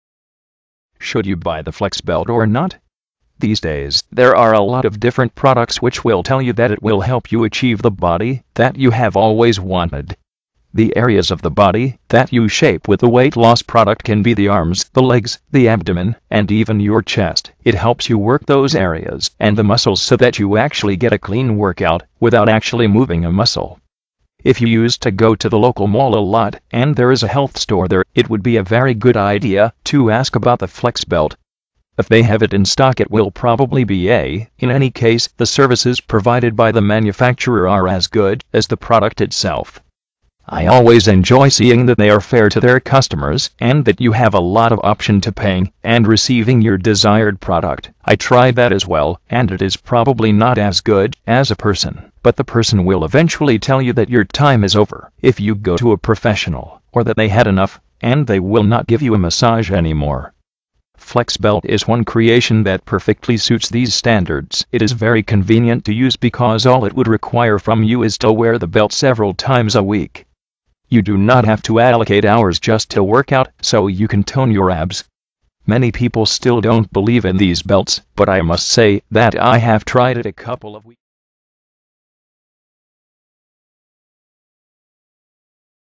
Wonderful Abs Not having Exercise
The way it functions is in the kind of a belt that incorporates gel pads and electrical manipulation of the abdominal muscles to "fool" the muscles into believing they are becoming exercised. Because the muscles are expanded and contracted, it has the same effect as in the event the user was doing sit-ups or stomach crunches.

Rowena A. Trevino